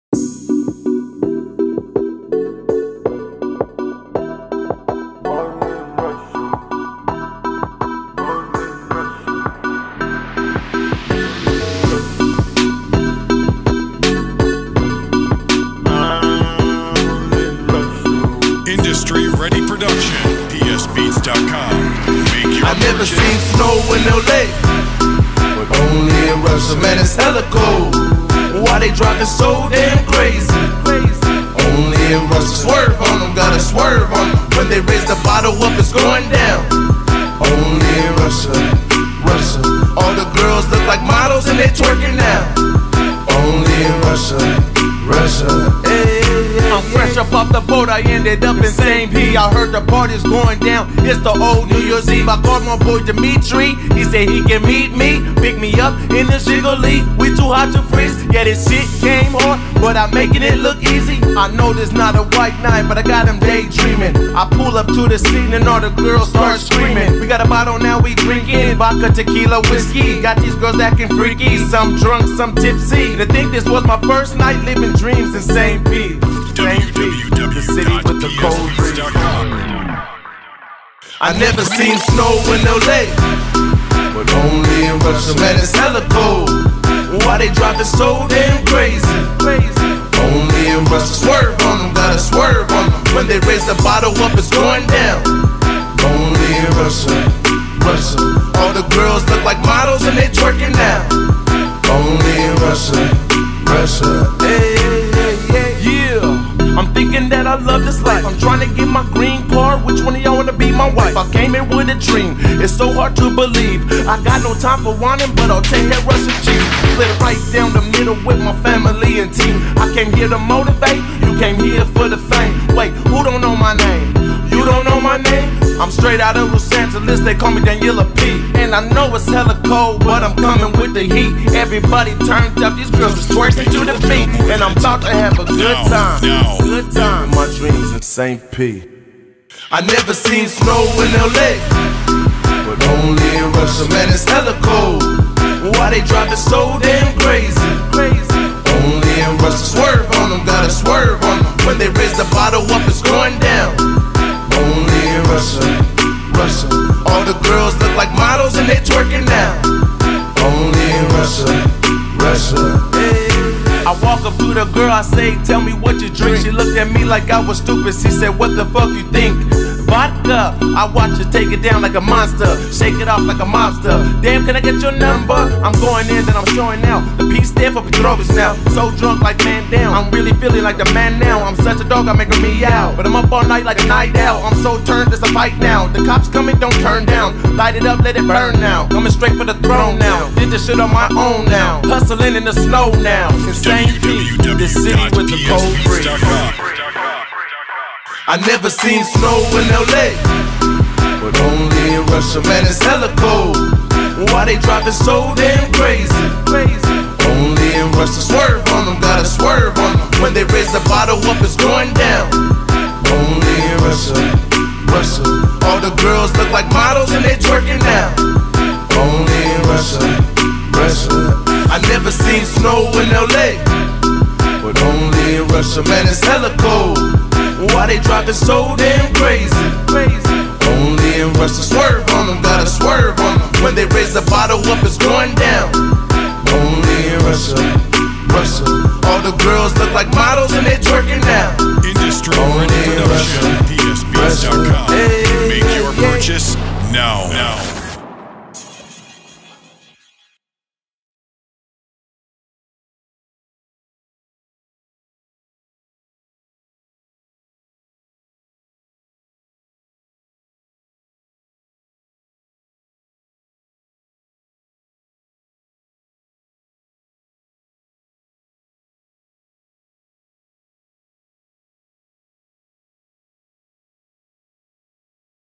Only In Russia by Danila Petrovich
Only In Russia is finally here ! Made in saint petersburg Russia.
after almost 8 months in russia Danila is going back home .but leaves his best project in Russia with LOVE ill be back
Special thank you to all of my friends and fans